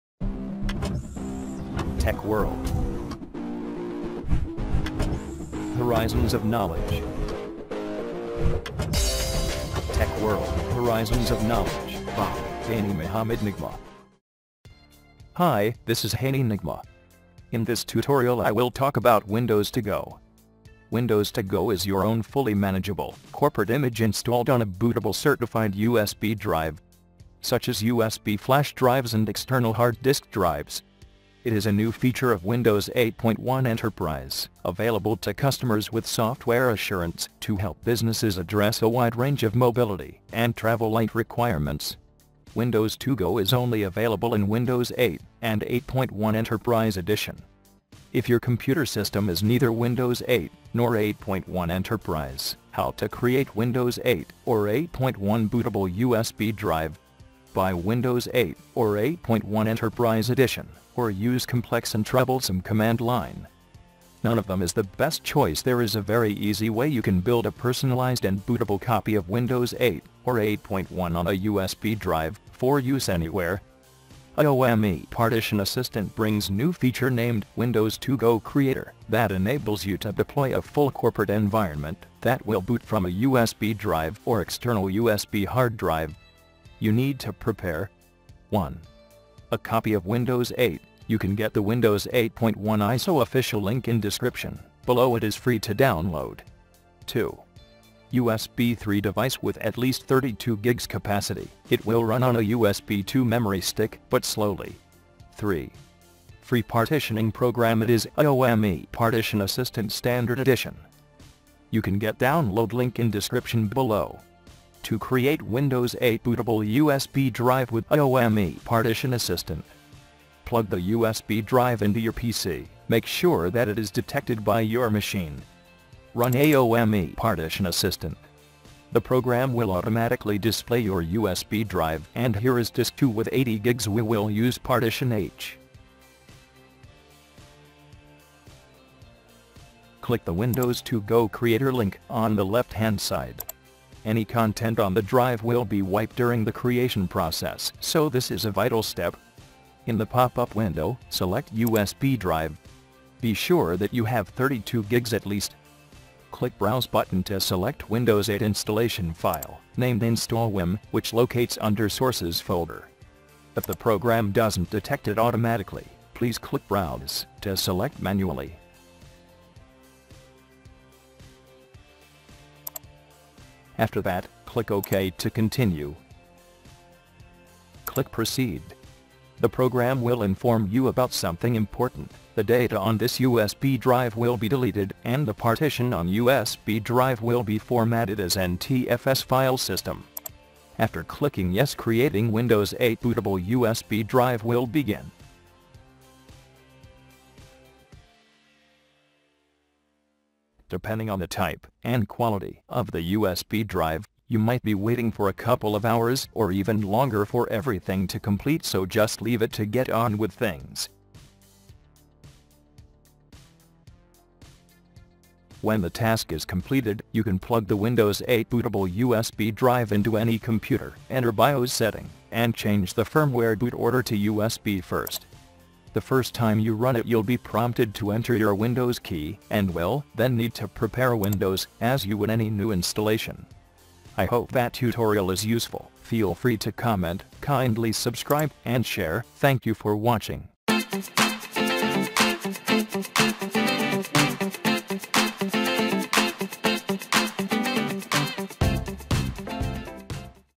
How to Create "Windows To Go" on a USB Drive
Windows to go is your own fully manageable, corporate image installed on a bootable certified USB drive. Such as USB flash drives and external hard disk drives It is a new feature of Windows 8.1 Enterprise
Available to customers with Software Assurance to help businesses address a wide range of mobility and travel light requirements.

There is a very easy way you can build a personalized and bootable copy of Windows 8 or 8.1 on a USB drive, for use anywhere

AOMEI Partition Assistant brings new feature named Windows To Go Creator that enables you to deploy a full corporate environment that will boot from a USB drive or external USB hard drive.

You Need to Prepare
1. a copy of Windows 8
2.USB3 device with at least 32 gigs capacity (it will run on a USB2 memory stick, but slowly)
3.free partitioning program it is AOMEI Partition Assistant Standard Edition

to Create Windows 8 Bootable USB Drive with AOMEI Partition Assistant

* Plug the USB drive into your PC  , Make sure that it is detected by your machine.

* Run AOMEI Partition Assistant . The program will automatically display your USB drive, and here is Disk 2 with 80 gigs we will use partition H: .

* click the Windows To Go Creator link on the left hand side

Any content on the drive will be wiped during the creation process so this is a vital step

In the pop-up window, select USB drive. Be sure that you have 32 gigs at least.

Click "Browse" button to select Windows 8 installation file (named install.wim) which locates under \sources folder. If the program doesn't detect it automatically, please click "Browse" to select manually. After that, click "OK" to continue.

Click "Proceed". The program will inform you about something important. The data on this USB drive will be deleted and the partition on USB drive will be formatted as NTFS file system.

After clicking "Yes", creating Windows 8 bootable USB drive will begin.

Depending on the type (and quality) of the USB drive, you might be waiting for a couple of hours or even longer for everything to complete so just leave it to get on with things.

When the task is completed, you can plug the Windows 8 bootable USB drive into any computer, , enter BIOS setting, and change the firmware boot order to USB first

The first time you run it you'll be prompted to enter your Windows key and will then need to prepare Windows as you would any new installation

I hope that tutorial is useful ,

Hi, this is Hany Nigma.
In this tutorial I Will talk about windows TO GO .

Windows to go is your own fully manageable, corporate image installed on a bootable certified USB drive. Such as USB flash drives and external hard disk drives

 It is a new feature of Windows 8.1 Enterprise

Windows To Go is only available in Windows 8 and 8.1 Enterprise Edition. If your computer system is neither Windows 8 nor 8.1 Enterprise, how to create Windows 8 or 8.1 bootable USB drive? Buy Windows 8 or 8.1 Enterprise Edition, or use complex and troublesome command line? None of them is the best choice!

مع تحياتي هاني محمد نجمه
تواصل معي على تواصل الاجتماعي :